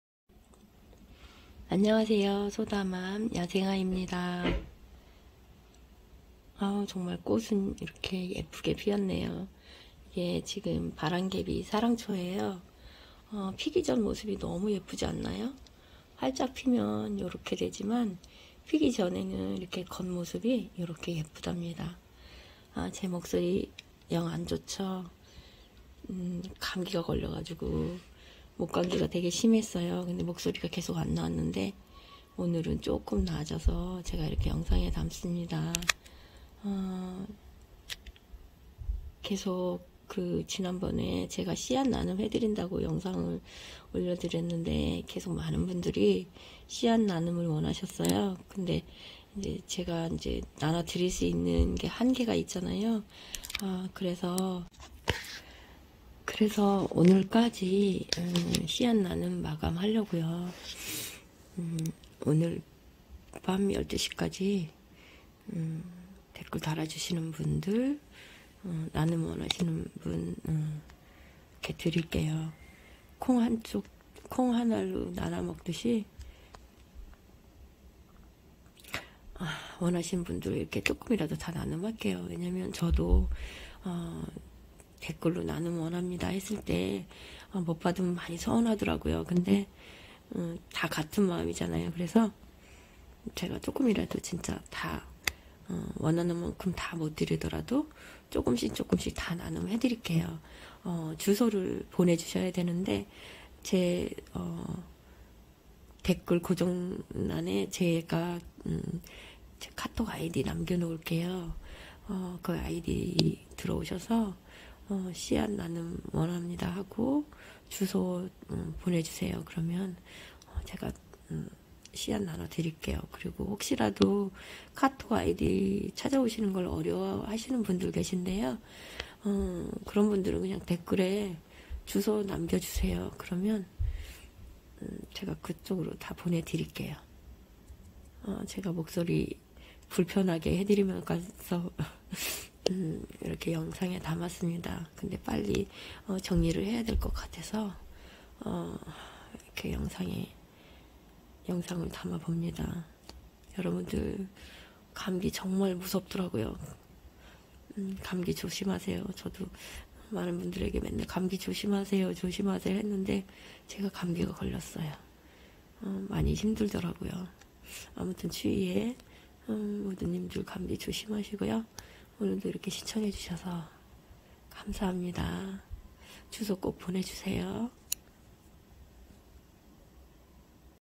씨앗나눔 오늘로 마감할께요
오늘밤12시까지할께요 가능한 조금씩 이라도 보내드리도록할께요